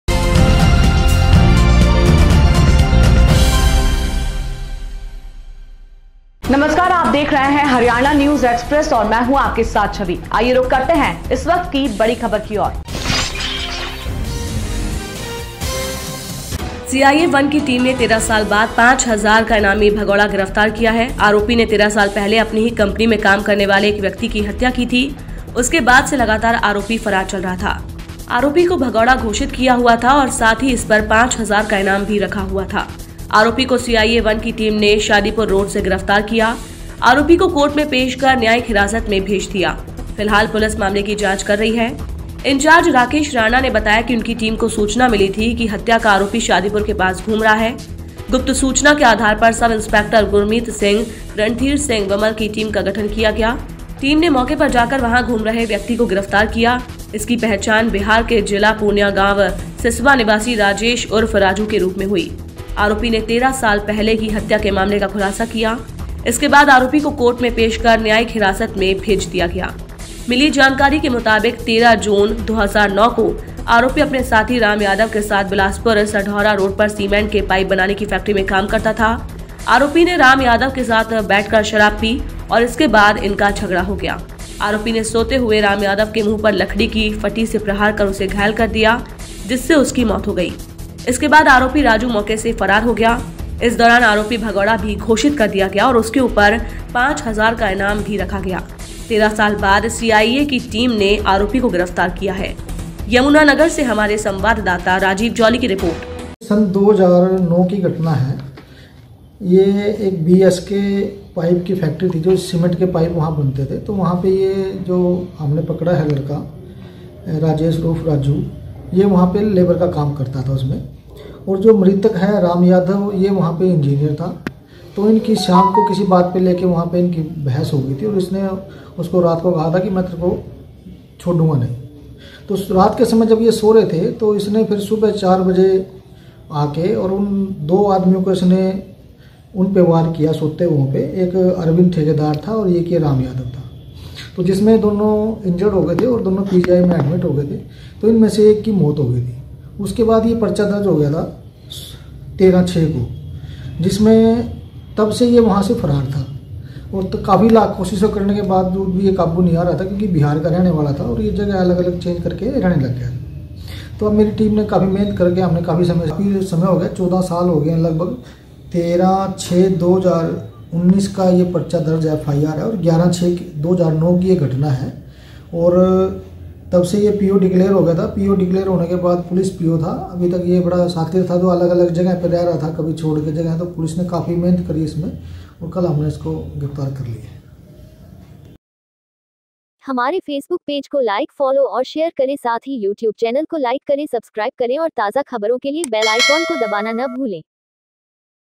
यमुनानगर : हत्या का इनामी आरोपी 13 साल बाद गिरफ्तार
सीआईए वन की टीम ने 13 साल बाद 5 हजार का इनामी भगोड़ा गिरफ्तार किया है। आरोपी ने 13 साल पहले अपनी ही कंपनी में काम करने वाले एक व्यक्ति की हत्या की थी। उसके बाद से लगातार आरोपी फरार चल रहा था। आरोपी को भगोड़ा घोषित किया हुआ था और साथ ही इस पर 5 हजार का इनाम भी रखा हुआ था। आरोपी को सीआईए वन की टीम ने शादीपुर रोड से गिरफ्तार किया। आरोपी को कोर्ट में पेश कर न्यायिक हिरासत में भेज दिया। फिलहाल पुलिस मामले की जांच कर रही है। इंचार्ज राकेश राणा ने बताया कि उनकी टीम को सूचना मिली थी कि हत्या का आरोपी शादीपुर के पास घूम रहा है। गुप्त सूचना के आधार पर सब इंस्पेक्टर गुरमीत सिंह, रणधीर सिंह विमल की टीम का गठन किया गया। टीम ने मौके पर जाकर वहां घूम रहे व्यक्ति को गिरफ्तार किया। इसकी पहचान बिहार के जिला पूर्णिया गांव सिसवा निवासी राजेश उर्फ राजू के रूप में हुई। आरोपी ने 13 साल पहले की हत्या के मामले का खुलासा किया। इसके बाद आरोपी को कोर्ट में पेश कर न्यायिक हिरासत में भेज दिया गया। मिली जानकारी के मुताबिक 13 जून 2009 को आरोपी अपने साथी राम यादव के साथ बिलासपुर साढौरा रोड पर सीमेंट के पाइप बनाने की फैक्ट्री में काम करता था। आरोपी ने राम यादव के साथ बैठकर शराब पी और इसके बाद इनका झगड़ा हो गया। आरोपी ने सोते हुए राम यादव के मुंह पर लकड़ी की फटी से प्रहार कर उसे घायल कर दिया जिससे उसकी मौत हो गई। इसके बाद आरोपी राजू मौके से फरार हो गया। इस दौरान आरोपी भगोड़ा भी घोषित कर दिया गया और उसके ऊपर 5 हजार का इनाम भी रखा गया। 13 साल बाद सीआईए की टीम ने आरोपी को गिरफ्तार किया है।